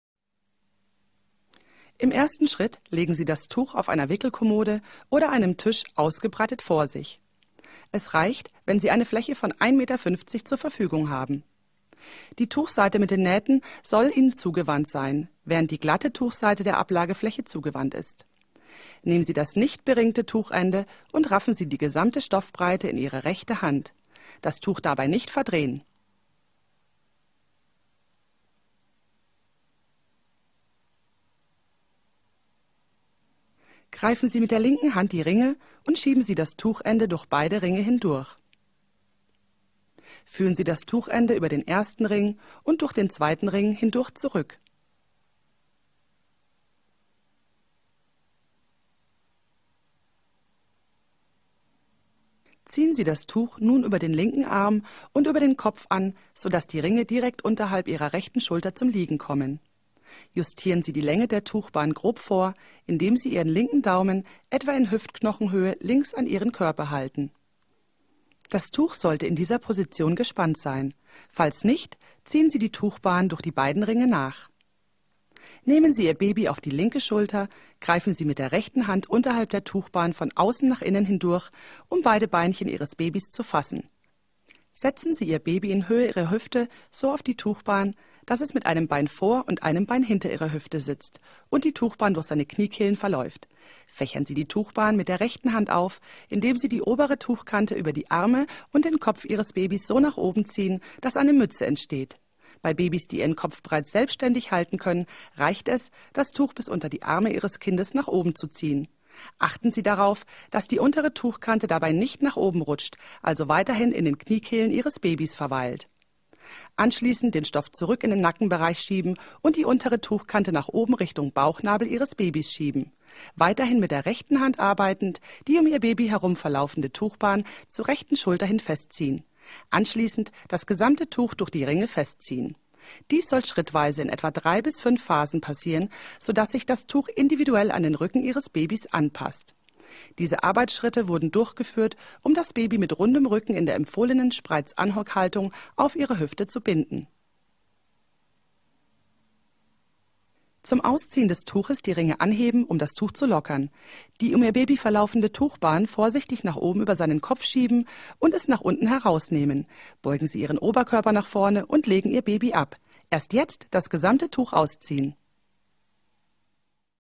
AMAZONAS Ring Sling
Οδηγίες δεσίματος μάρσιπος Ring Sling.